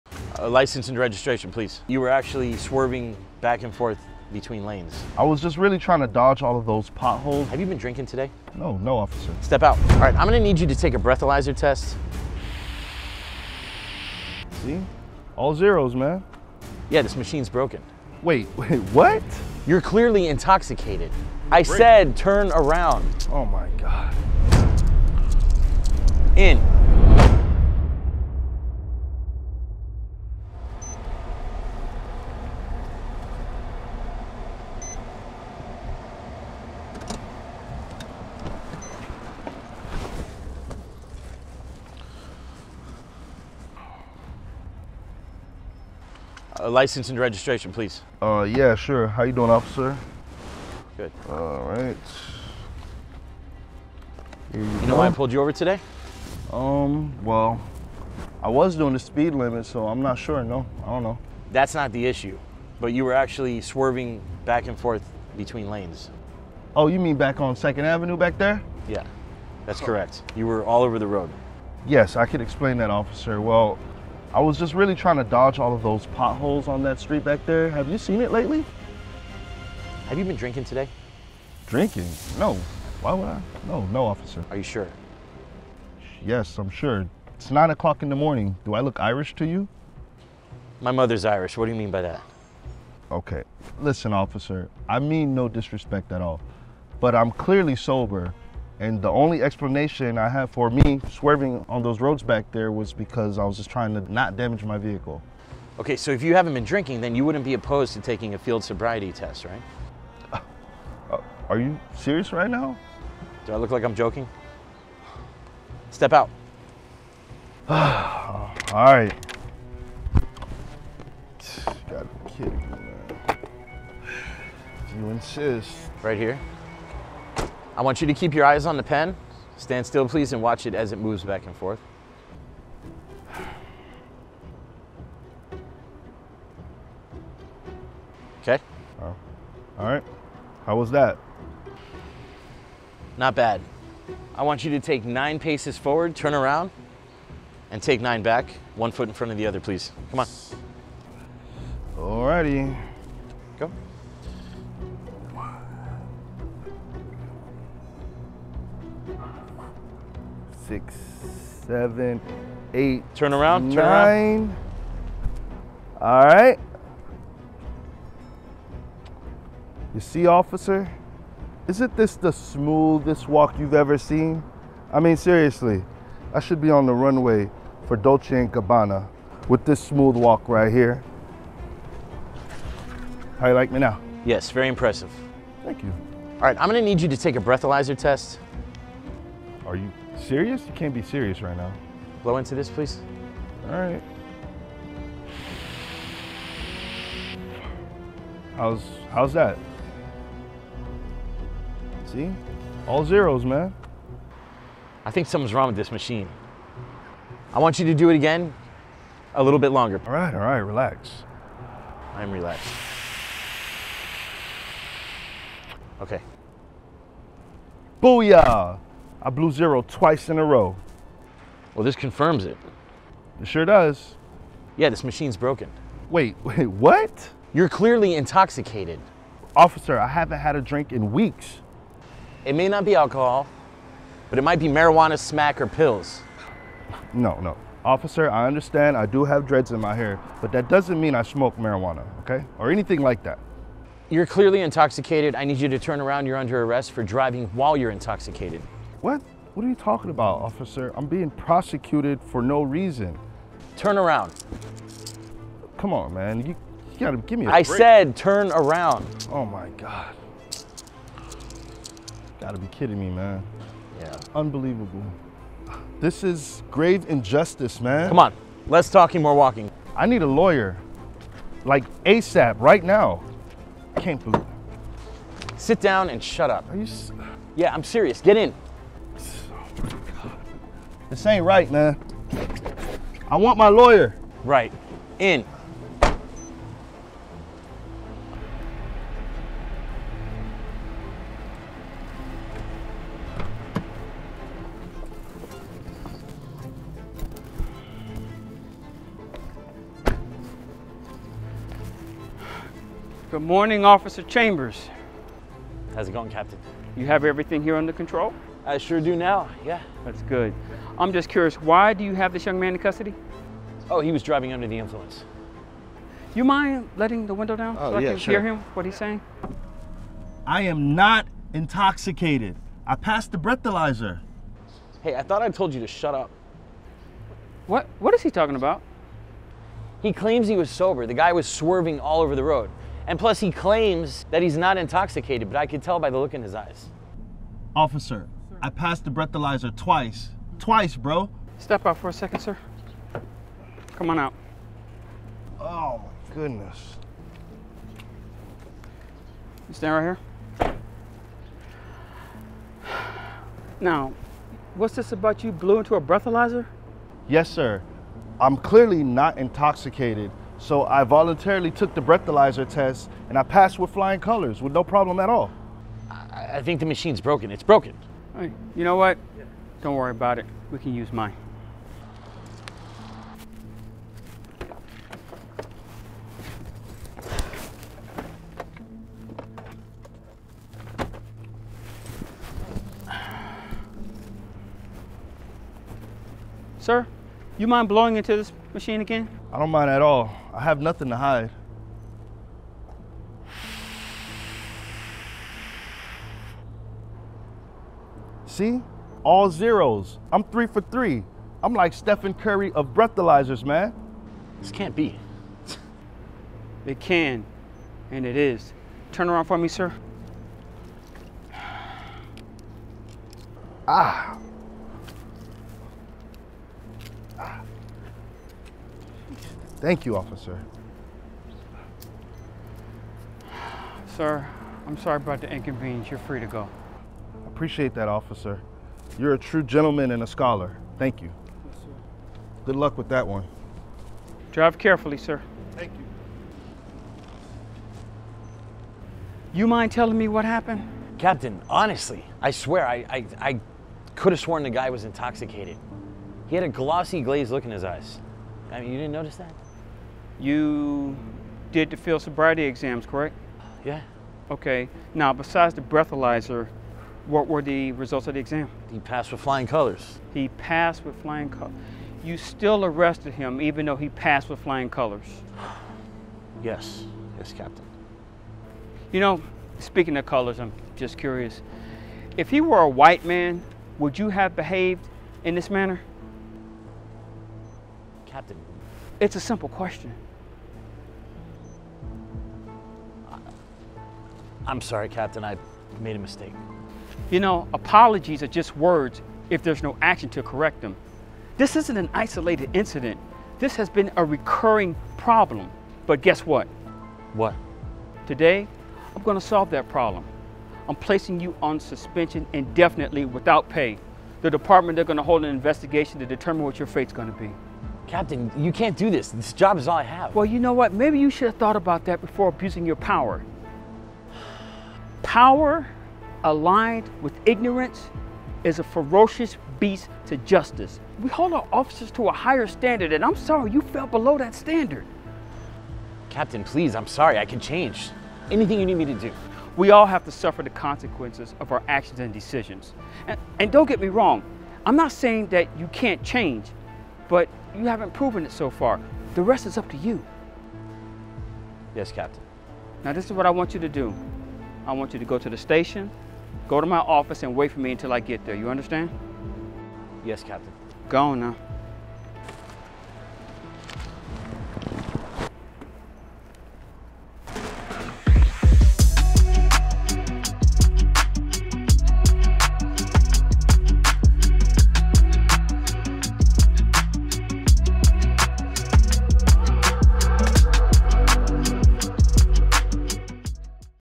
Police Falsely Arrests Black Man Doing Nothing Wrong. Then This Happens
Mango Park Police Officer Chambers pulls over a motorist for swerving in between lanes. When he approaches the driver, the officer suspects that he has been drinking. The driver denies the accusation and consents to taking a field sobriety test. The driver passes both the field and breathalyzer tests, but that doesn't stop Officer Chambers from arresting him anyway.  Unfortunately, for Officer Chambers when backups arrives, Captain James Porter righteously lets the innocent man free.

Starring
Verne Alexandre
Seth Harris Gordon
Ricky Rolez